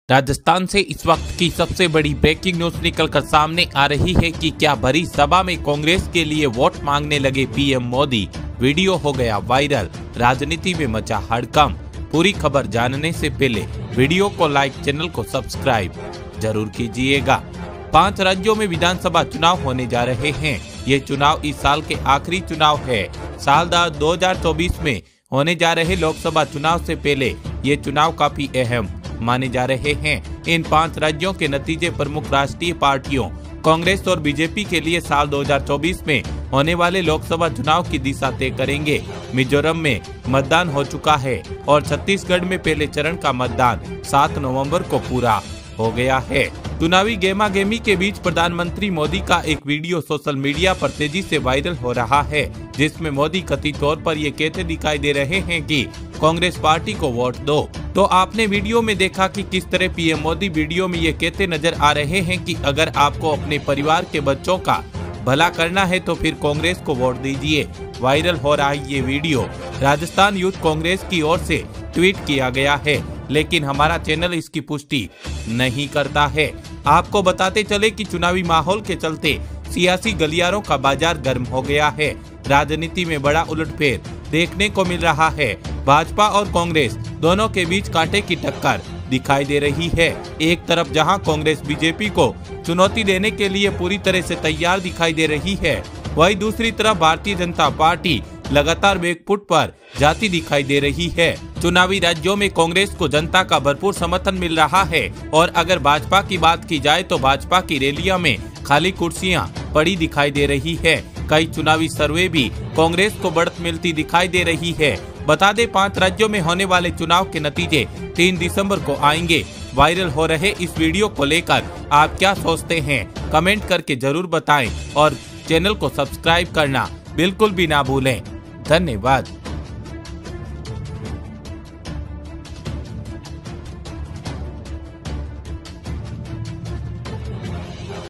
रैली के दौरान पीएम मोदी कांग्रेस के लिए मांग रहे वोट सचिन को बताया सीएम ! बड़ी खबर
News24RjHindi.